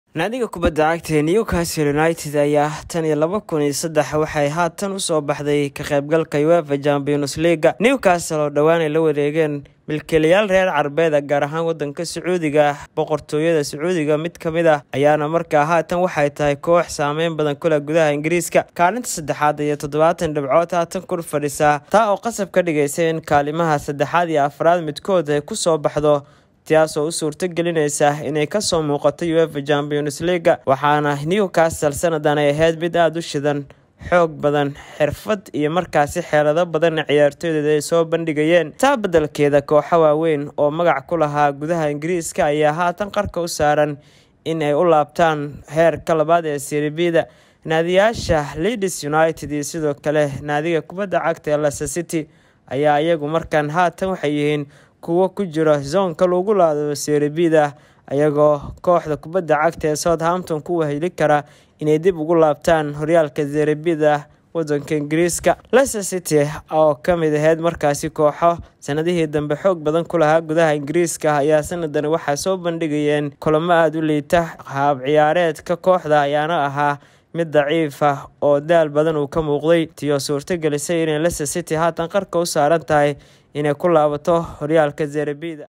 Newcastle United 20sano Kahor Ayaa Ugu Dambeesay Inay Cayaarto Champions League
Newcastle United 20sano Kahor Ayaa Ugu Dambeesay Inay Cayaarto Champions League Halka Leicester Ay U Dhowdahy Inay Laabato.